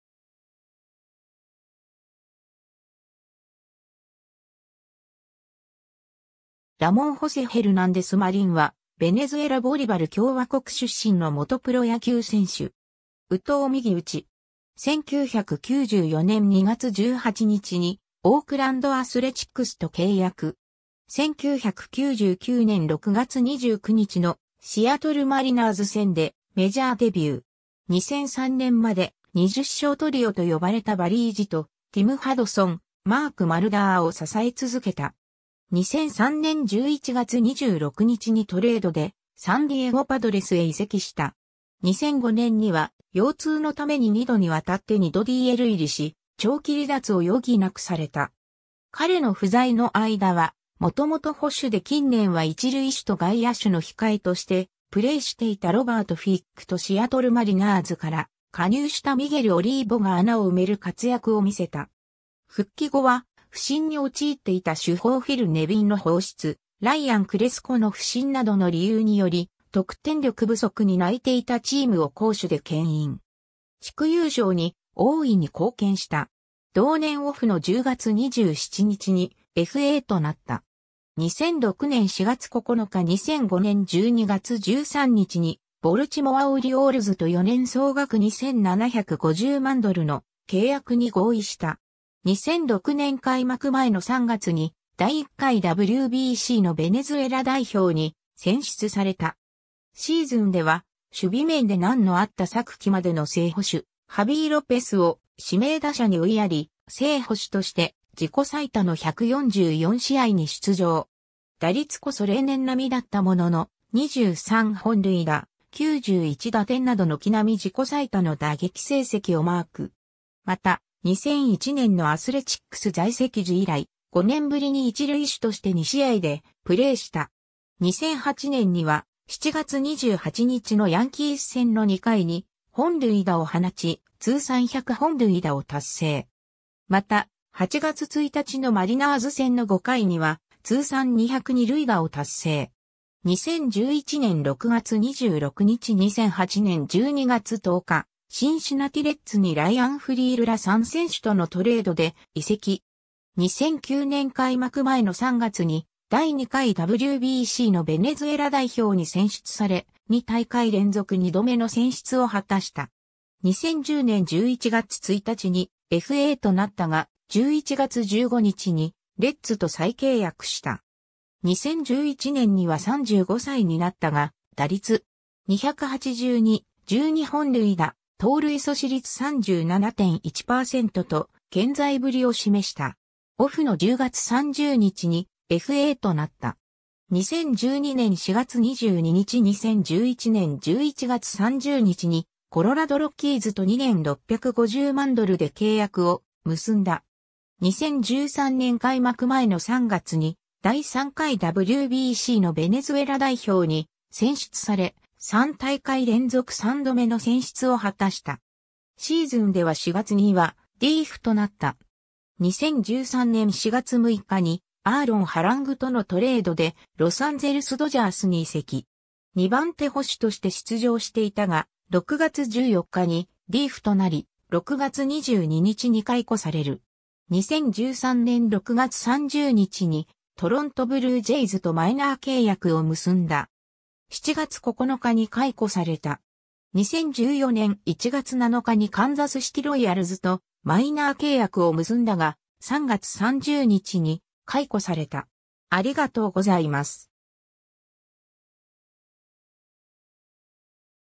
ラモン・ヘルナンデス
ラモン・ホセ・ヘルナンデス・マリン（Ramón José Hernández Marin, 1976年5月20日 - ）は、ベネズエラ・ボリバル共和国出身の元プロ野球選手（捕手）。
右投右打。
 1994年2月18日にオークランド・アスレチックスと契約。
 1999年6月29日のシアトル・マリナーズ戦でメジャーデビュー。
 2003年まで、20勝トリオと呼ばれたバリー・ジト、ティム・ハドソン、マーク・マルダーを支え続けた。
 2003年11月26日にトレードでサンディエゴ・パドレスへ移籍した。
 2005年には、腰痛のために2度にわたって2度DL入りし、長期離脱を余儀なくされた。
彼の不在の間は、元々捕手で近年は一塁手と外野手の控えとしてプレーしていたロバート・フィックとシアトル・マリナーズから加入したミゲル・オリーボが穴を埋める活躍を見せた。
復帰後は、不振に陥っていた主砲フィル・ネビンの放出、ライアン・クレスコの不振などの理由により、得点力不足に泣いていたチームを攻守で牽引。
地区優勝に大いに貢献した。
同年オフの10月27日にFAとなった。
 2006年4月9日 2005年12月13日にボルチモア・オリオールズと4年総額2750万ドルの契約に合意した。
 2006年開幕前の3月に第1回WBCのベネズエラ代表に選出された。
 シーズンでは、守備面で難のあった昨季までの正捕手ハビー・ロペスを指名打者に追いやり、正捕手として自己最多の144試合に出場。
打率こそ例年並みだったものの、23本塁打・91打点など軒並み自己最多の打撃成績をマーク。
また、2001年のアスレチックス在籍時以来、5年ぶりに一塁手として2試合でプレーした（すべて途中出場で5イニング、5度の守備機会で無失策）。
 2008年には、7月28日のヤンキース戦の2回に本塁打を放ち、通算100本塁打を達成。
また、8月1日のマリナーズ戦の5回には通算200二塁打を達成。
 2011年6月26日 2008年12月10日、シンシナティ・レッズにライアン・フリールら3選手とのトレードで移籍。
 2009年開幕前の3月に第2回WBCのベネズエラ代表に選出され、2大会連続2度目の選出を果たした。
 2010年11月1日にFAとなったが、11月15日にレッズと再契約した。
 2011年には35歳になったが、打率.282、12本塁打、盗塁阻止率37.1％と健在ぶりを示した。
 2012年4月22日 2011年11月30日にコロラド・ロッキーズと2年650万ドルで契約を結んだ。
 2013年開幕前の3月に第3回WBCのベネズエラ代表に選出され、3大会連続3度目の選出を果たした。
 シーズンでは4月にはDFAとなった。
 2013年4月6日にアーロン・ハラングとのトレードでロサンゼルス・ドジャースに移籍。
2番手捕手として出場していたが、6月14日にDFAとなり、6月22日に解雇される。
 2013年6月30日にトロント・ブルージェイズとマイナー契約を結んだ。
7月9日に解雇された。
 2014年1月7日にカンザスシティ・ロイヤルズとマイナー契約を結んだが、3月30日に解雇された。